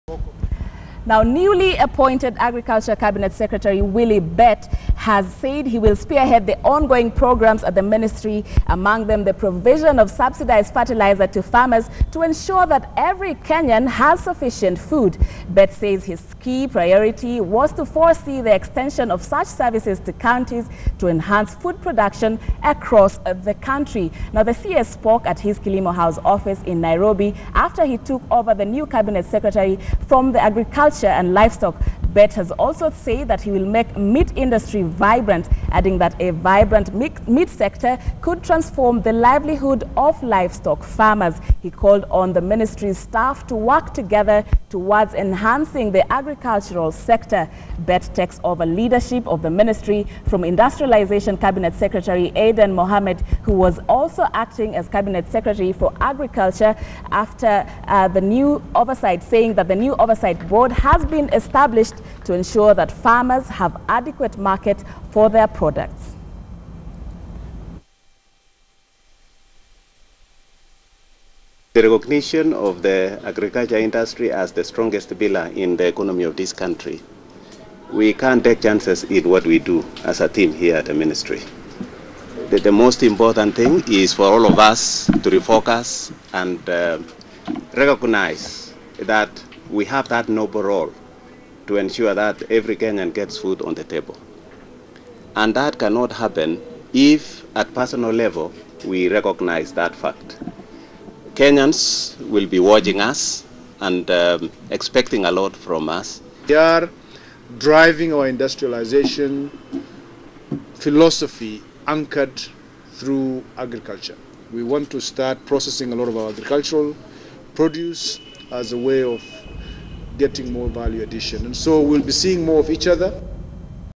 Seven Cabinet Secretaries and 24 PSS join the executive amide promises to fight graft
Keter, the new Energy and Petroleum Cabinet Secretary who until his appointment was the Deputy Majority Leader will now be replaced by Elgeyo/Marakwet Senator Kipchumba Murkomen.